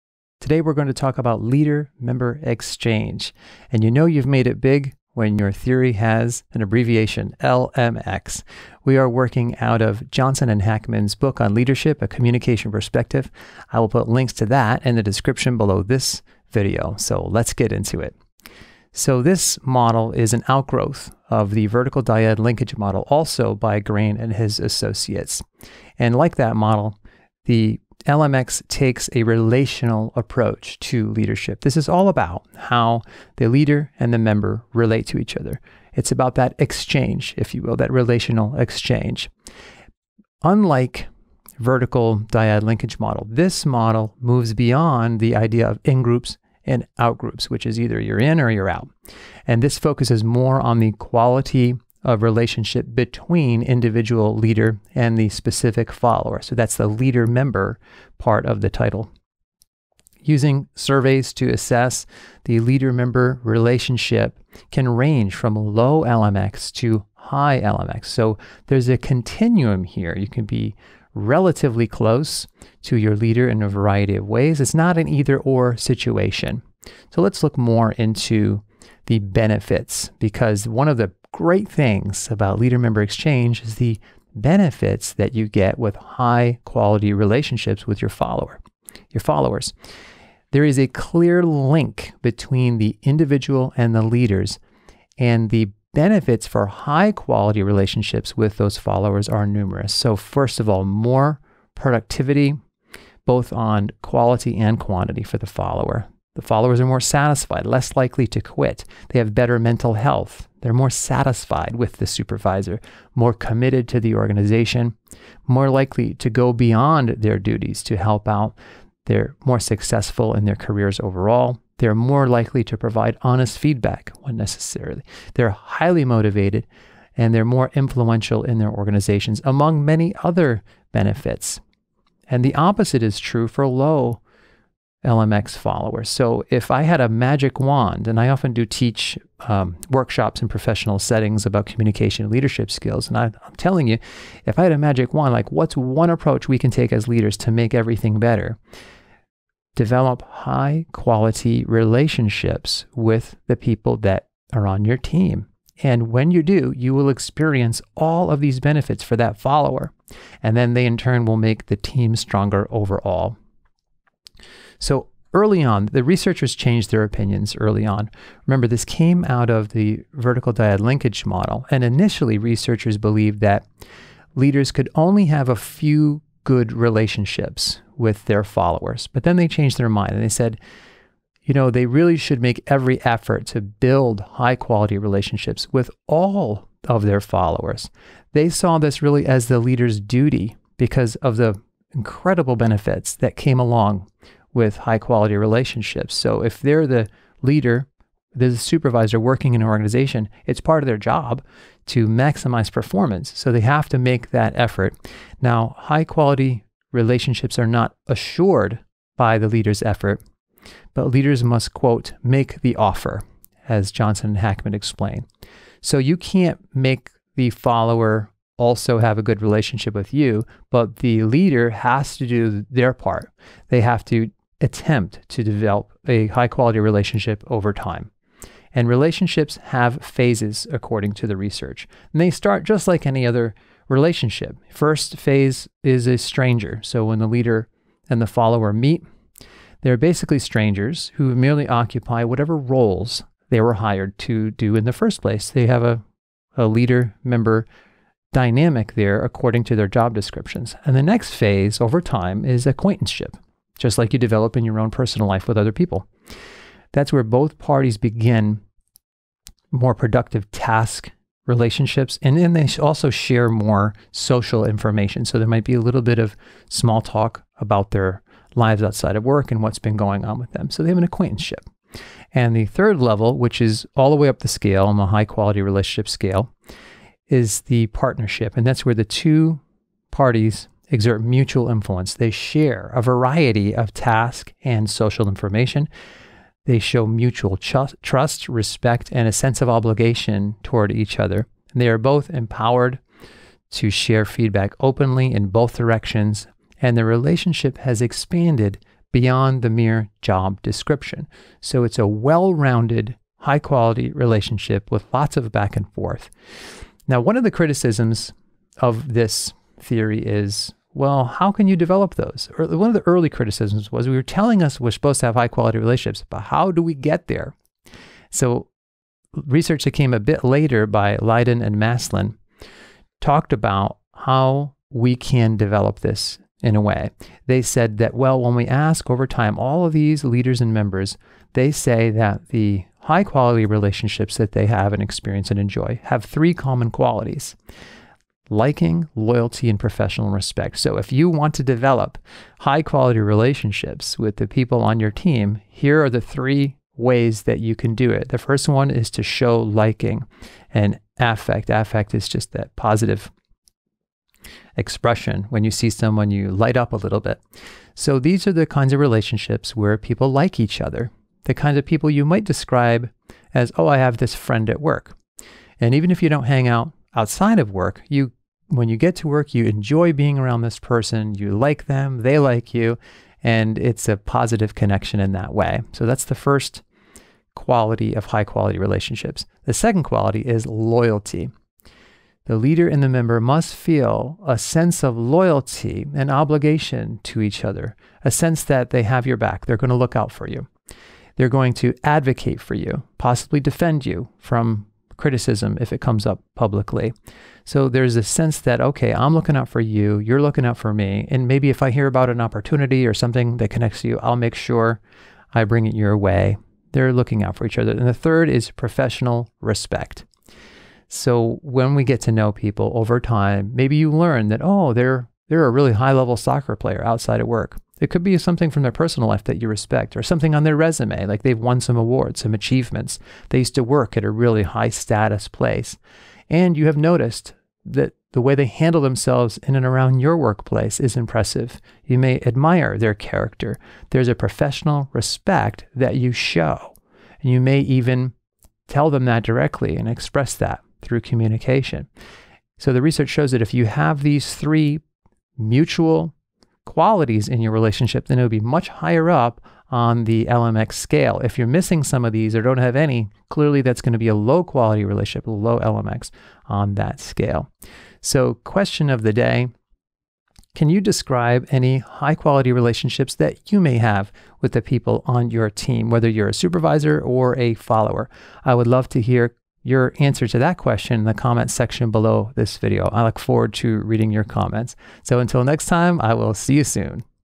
Leader Member Exchange Theory
Leader-Member Exchange Theory (LMX) is a powerful area of leadership research because it shows a direct connection from the quality of the leader-member relationship to the productivity, satisfaction, initiative of followers, and many other beneficial outcomes. Few other areas of research can successfully show the quantifiable benefits like this.

Leader-Member Exchange has its roots in a related theory, Vertical Dyad Linkage Theory, a model by the same group of researchers, George B. Graen and associates.

The Organizational Communication Channel provides supplemental lessons for traditional, hybrid, and 100% online courses for students and teachers everywhere.